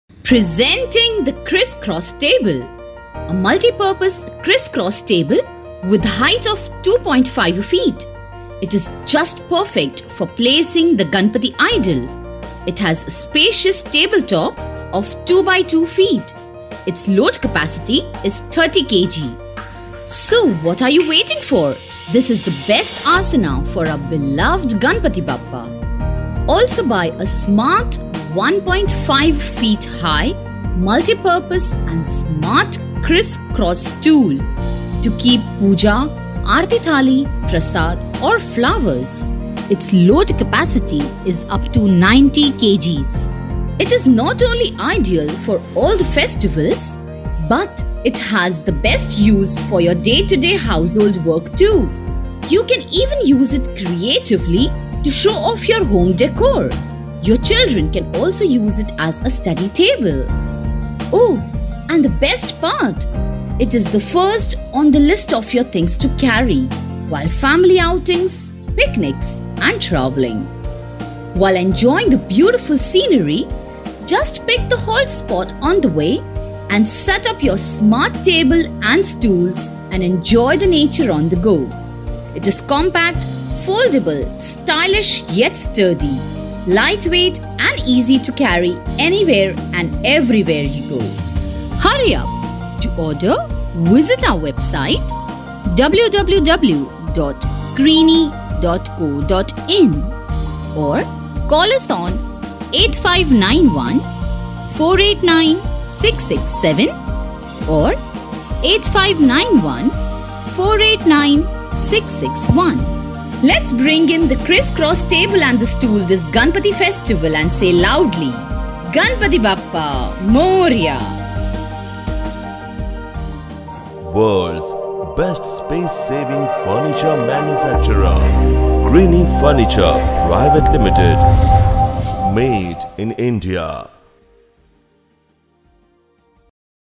Foldable Table | Outdoor Table | Portable Table | Camping Table
Foldable Table by Krini is a functional product that offers you convenience no matter where you go.

Folding tables can come in handy when you have a social gathering at home. You can use these tables whenever there is a requirement and fold them back when they are not in use.

It is a space-saving option when used at home as it can be stored conveniently after use. It is a practical choice which comes in useful if you travel a lot. It is a great option for outdoor events too because of its lightweight.

We at Krini Furniture, believe to provide good quality products to our customer so that they will get satisfaction as they want.